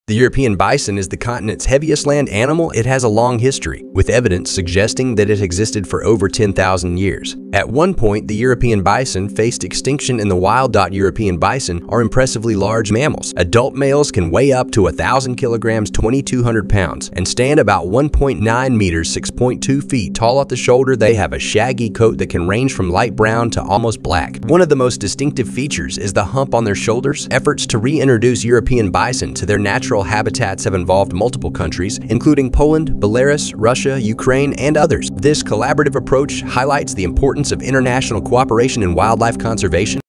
"Conservation Chronicles: Saving Europe's Bison"
1. **Ancient Heritage:** The European bison is the continent's heaviest land animal and is considered a living relic from prehistoric times. It has a long history, with evidence suggesting that it has existed for over 10,000 years.

2. **Endangered Status:** At one point, the European bison faced extinction in the wild. However, through conservation efforts, particularly in Poland and Belarus, their numbers have increased, and they are no longer classified as critically endangered. Still, they remain a vulnerable species.

3. **Majestic Size:** European bison are impressively large mammals. Adult males can weigh up to 1,000 kilograms (2,200 pounds) and stand about 1.9 meters (6.2 feet) tall at the shoulder.

4. **Distinctive Appearance:** They have a shaggy coat that can range from light brown to almost black. One of the most distinctive features is the hump on their shoulders, which is more pronounced in males than in females.

5. **Social Behavior:** European bison are social animals and form herds, typically led by a dominant female. During the mating season, dominant males compete for the right to mate with females, engaging in impressive displays of strength and dominance.

6. **Habitat Preferences:** They are adaptable creatures that can inhabit various environments, including forests, grasslands, and even marshes. Their diet consists mainly of grasses, herbs, and shrubs.

7. **Conservation Success:** The successful conservation of the European bison is a testament to the effectiveness of concerted conservation efforts. Various breeding and reintroduction programs have played a crucial role in saving the species.

8. **Symbolic Significance:** The European bison holds cultural and symbolic significance in many countries. It is often considered a symbol of nature conservation success and a connection to Europe's wild heritage.

9. **Reintroduction Efforts:** Efforts to reintroduce European bison to their natural habitats have involved multiple countries, including Poland, Belarus, Russia, Ukraine, and others. This collaborative approach highlights the importance of international cooperation in wildlife conservation.

10. **Genetic Diversity:** Maintaining genetic diversity is crucial for the long-term survival of the species. To prevent inbreeding, there are ongoing efforts to manage the European bison population carefully, including the exchange of individuals between different herds.